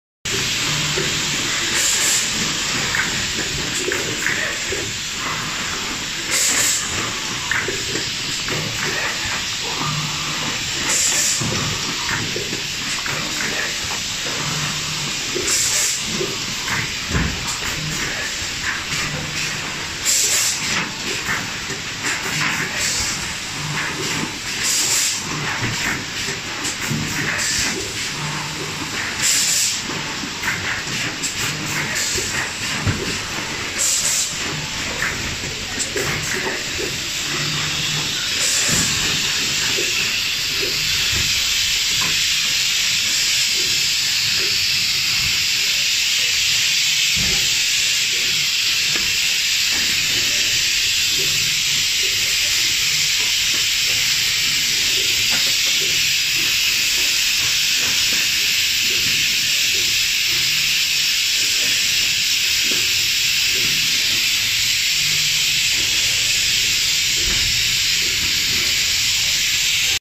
Semi-auto rigid box maker with box gripper
This machine is a upgrade model of BF540 rigid box wrapper. Its mechanical gripper can deliver the box from the conveyor to the molds. It could equip on the usual rigid box gluing machine without any modification. Pls contact Blake for more details.